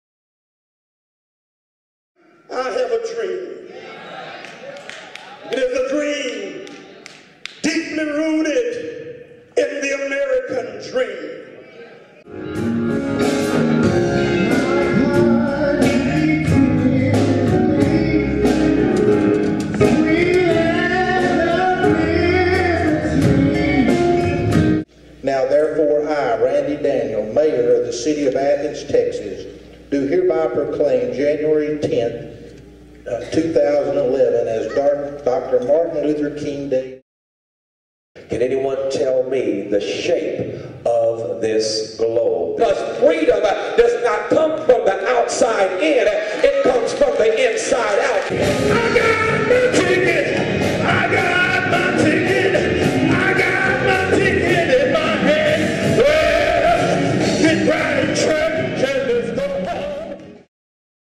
Trailer for Henderson County Celebrates Dr. Martin Luther King, Jr
This is the trailer for the two hour special, Henderson County Celebrates Dr. Martin Luther King, Jr.  It was filmed on January 10th at the Cain Center in Athens, TX.  Lots of signing, praising, messages and delivery of The I have a dream speech by Pastor Victorio Rockmore.